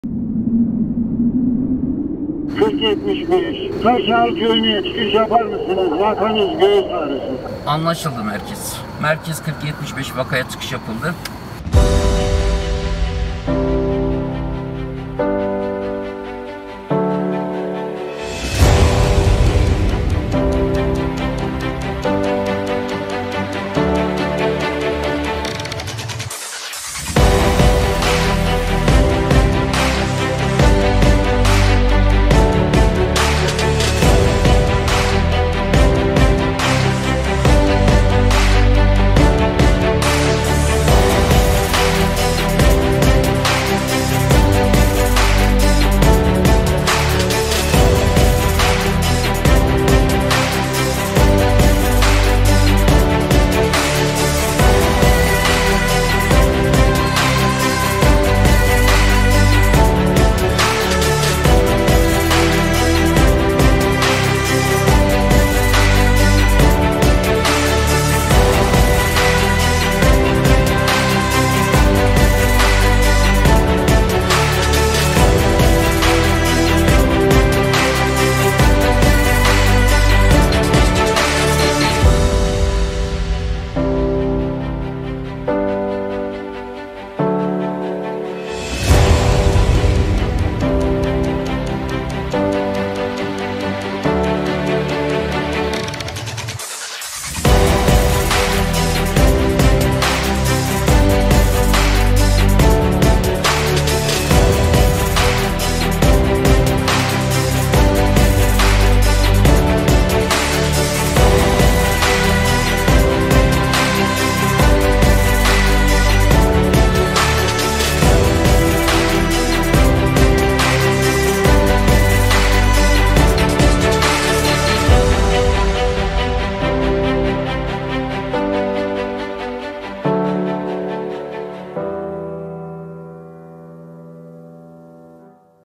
“Olmaya devlet cihânda bir nefes sıhhat gibi.” Milletimizin Hizmetindeyiz.
Erzurum’un Taşağıl köyündeki hastamıza sağlık ekiplerimizin müdahalesi